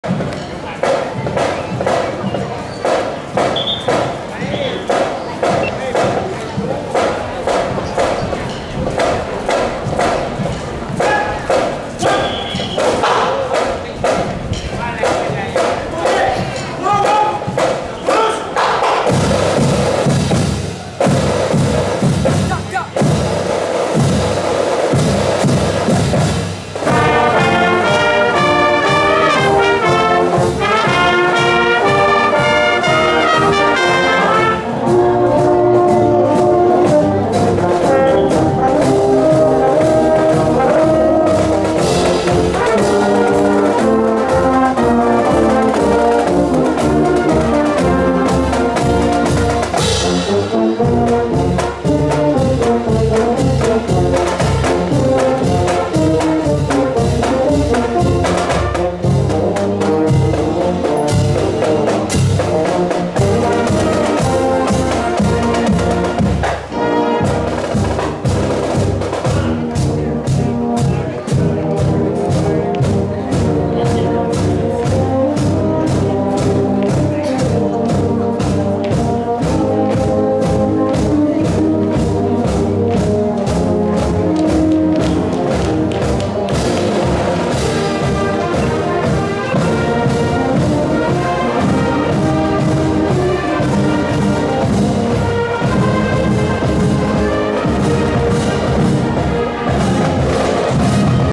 วงโยธวาทิต โรงเรียนสตรีสิริเกศ
ประกวดวงโยธวาทิต ชิงถ้วยพระราชทานฯ 2561
Thailand World Music Championships 2018